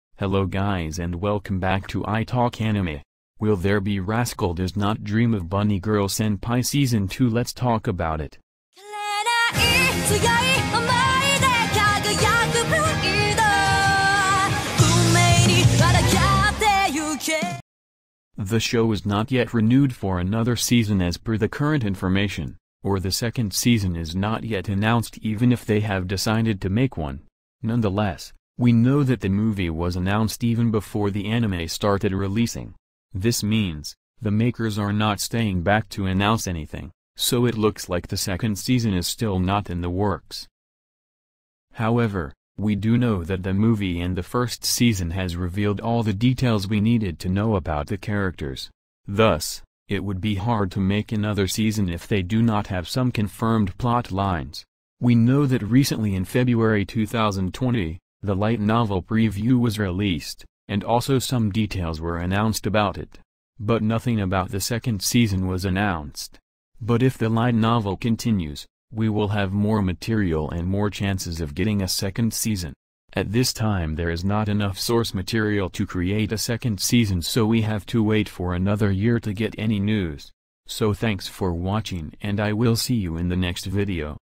Rascal Does Not Dream of Bunny Girl Senpai Season 2 Confirmed? | Release Date | Light Novel?
Rascal Does Not Dream of Bunny Girl Senpai, known in Japan under the general title Seishun Buta Yarō (Japanese: 青春ブタ野郎) and short name Aobuta (青ブタ), is a Japanese light novel series written by Hajime Kamoshida and illustrated by Keeji Mizoguchi. ASCII Media Works have published eleven volumes since April 2014 under their Dengeki Bunko imprint. A manga adaptation with art by Tsugumi Nanamiya has been serialized in ASCII Media Works' seinen manga magazine Dengeki G's Comic since December 2015. An anime television series adaptation of the first five series volumes by CloverWorks aired from October to December 2018. An anime film adaptation emphasizing series volumes six and seven titled Rascal Does Not Dream of a Dreaming Girl premiered in June 2019.